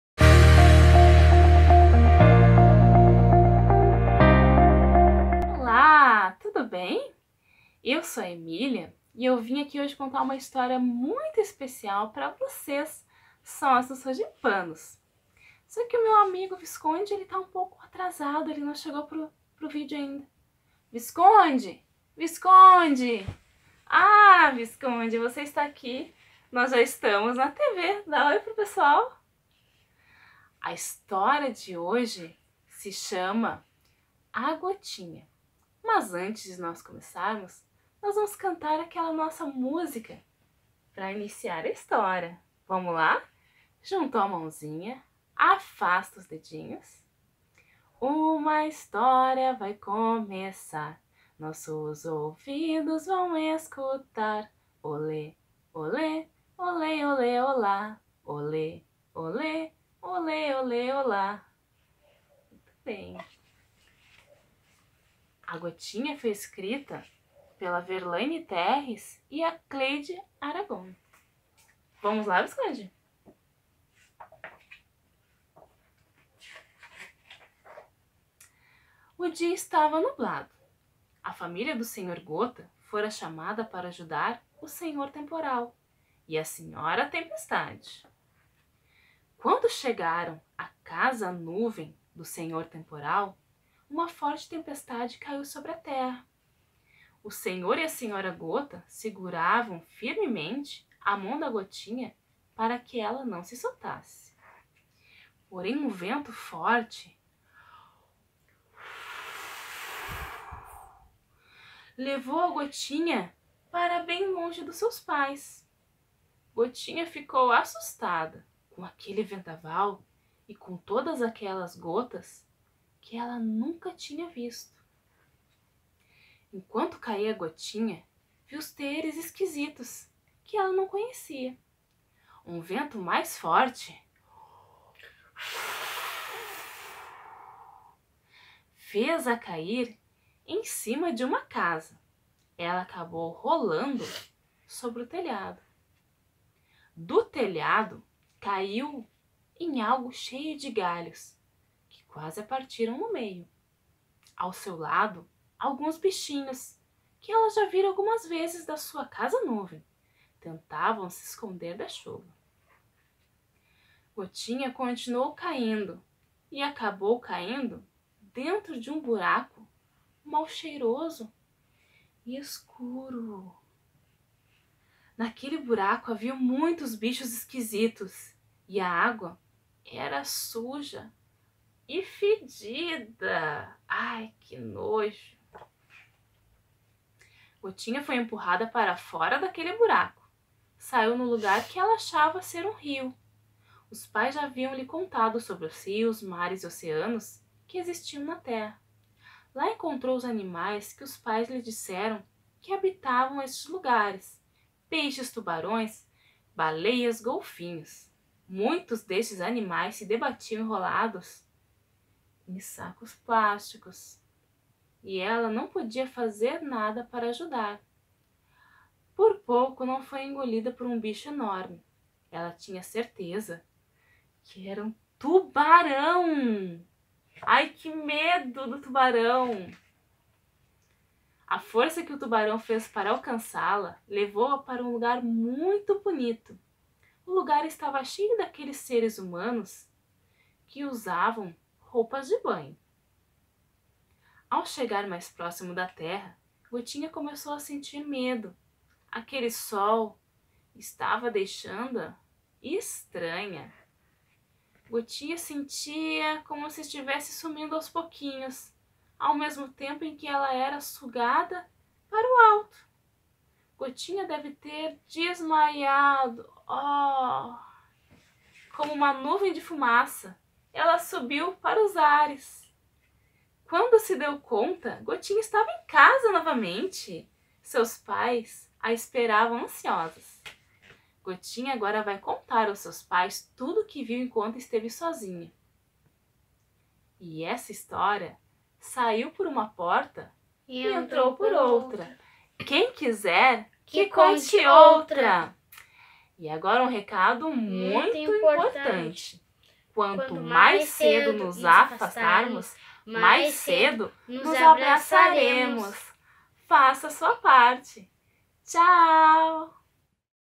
Sogipa em Casa - Contação de Histórias #13
A bibliotecária da Sogipa, Jacy Gonçalves, conta a história do livro "A Gotinha", escrito por Berlaine Terres e Cleide Aragón.